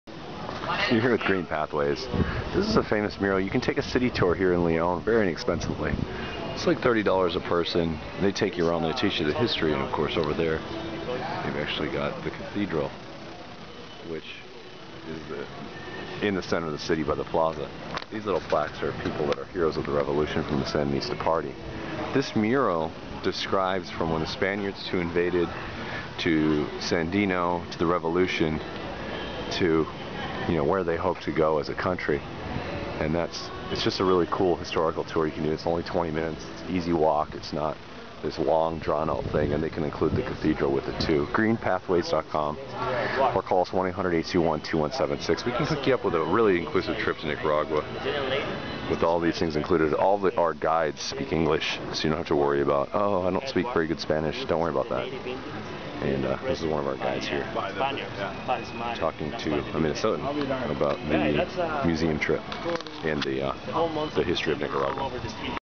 Nicaragua Tour Operator Side Trips - Leon Nicaragua
offers sustainable travel to nicaragua as well as green travel to Nicaragua. Here we have a video of the a famous mural in Leon Nicaragua about the history of Nicaragua. Contact Green Pathways for a super inclusive trip to Nicaragua.